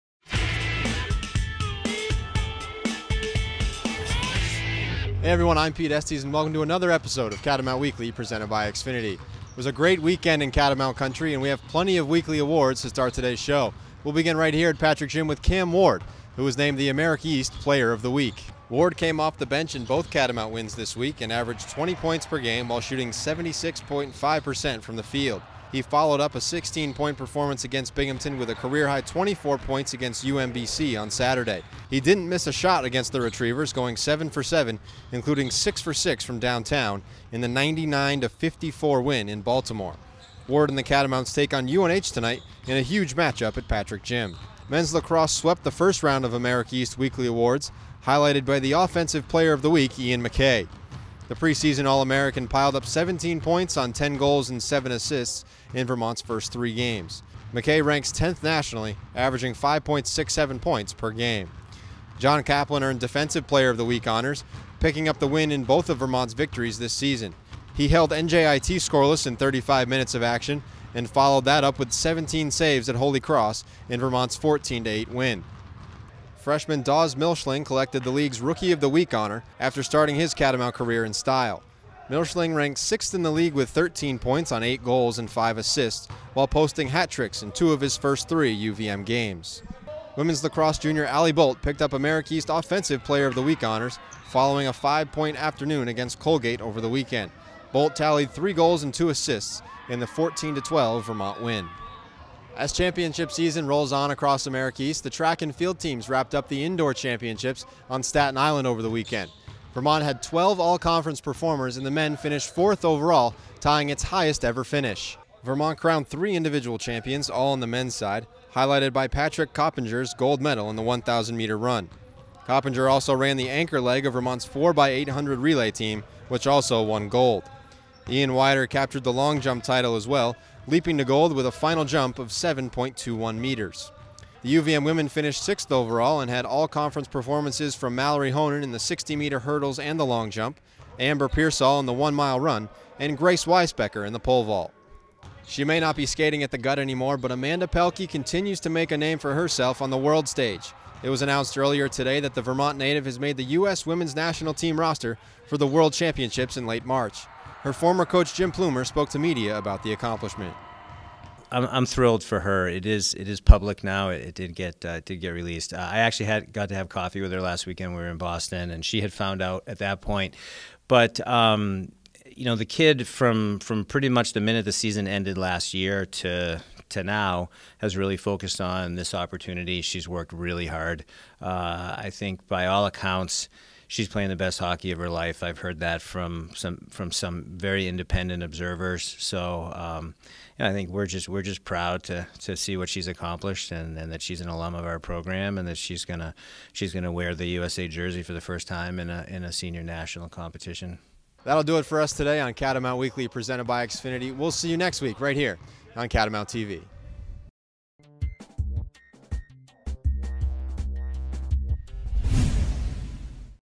Catamount Weekly Presented by XFINITY (2/24/16)
Ward Nets America East Honors: Cam Ward picked up America East Player of the Week Honors

Men's Lacrosse off to Strong Start: The men's lacrosse team swept the first round of America East weekly awards.

Bolt Strikes for UVM Women's Lacrosse: Women's lacrosse junior Alison Bolt is America East's offensive player of the week.

Indoor Track and Field Championships: The Catamounts had three champions and 12 all-conference performers at this past weekends indoor track and field championships.

Pelkey Named to World Championships Roster: Former captain Amanda Pelkey has been named to the U.S. Women's National Team that will compete in the 2016 IIHF Women's World Championship March 28-April 4, in Kamloops, British Columbia.